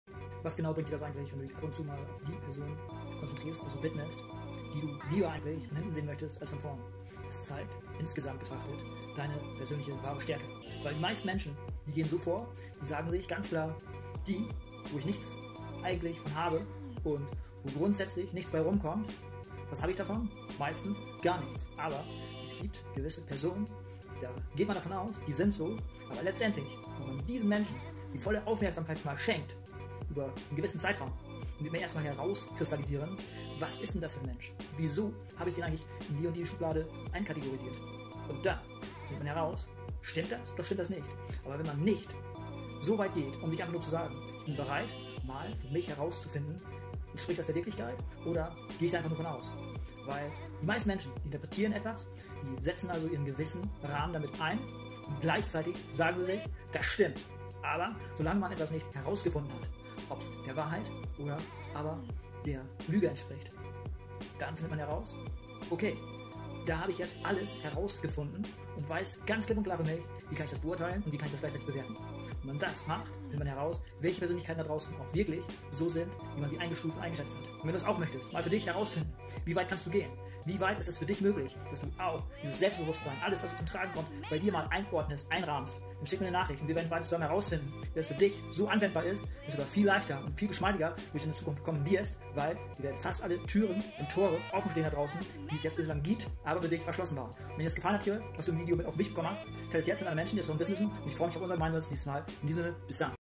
Warum sollten wir uns auf einen Menschen konzentrieren?
Widmest Du Dich noch immer Menschen, die Du in Wirklichkeit lieber von hinten sehen möchtest?

Zeigt Dir das dann Deine persönliche und wirkliche Stärke?

Warum schenkst Du diesen Personen bislang noch immer die volle Aufmerksamkeit? Finde besser mal für Dich heraus, ob das der wirklichen Wirklichkeit entspricht? Oder hast Du Dir mit dieser Vorgehensweise immer nur etwas eingeredet? Gehe also immer für Dich so weit, bis Du genau an diesem Punkt angelangt bist!

Schau Dir dazu zusätzlich dieses Video an!

Möchtest Du zusätzlich einen Unterschied herbeiführen?

Schick mir dazu einfach eine Nachricht oder besuche meine Seite! 👍